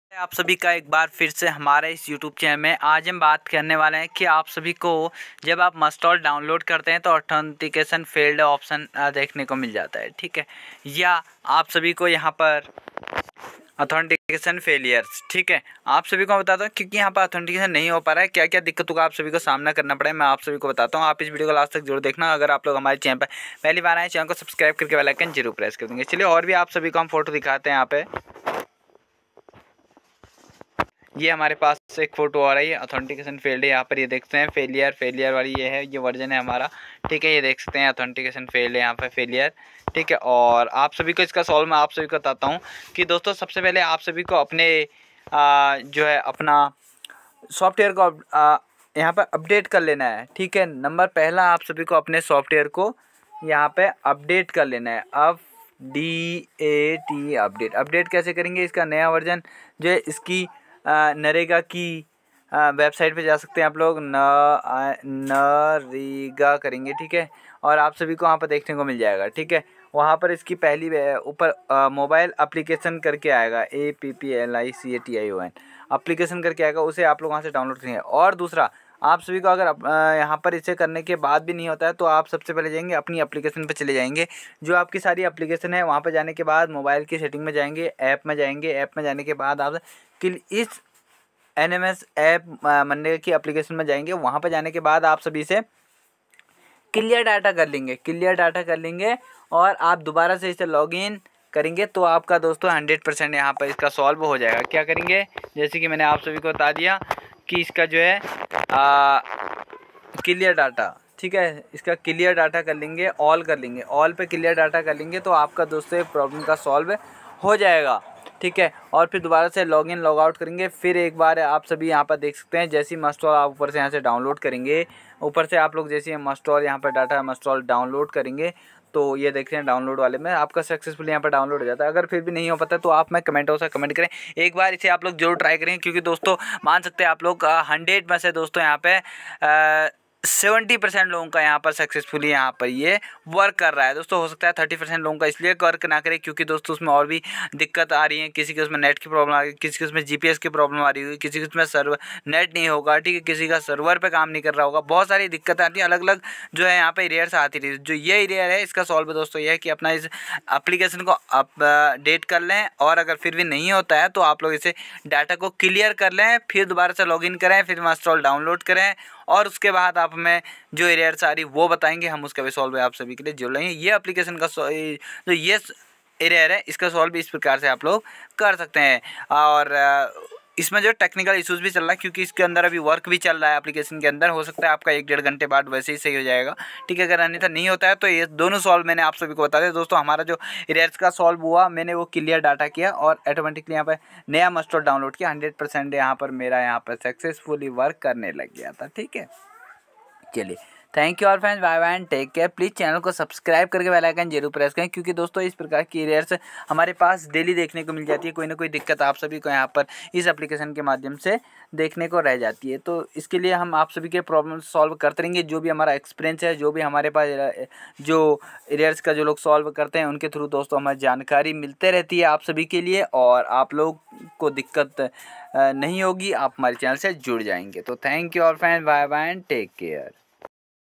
Authentication failure error ? how to fix this Problem,नरेगा श्रमिकों की हाजरी || Nmms app Error ❓
Authentication failure error ? how to fix this Problem,
नरेगा श्रमिकों की हाजरी
|| Nmms app Error ❓

technical bhattji YouTube channel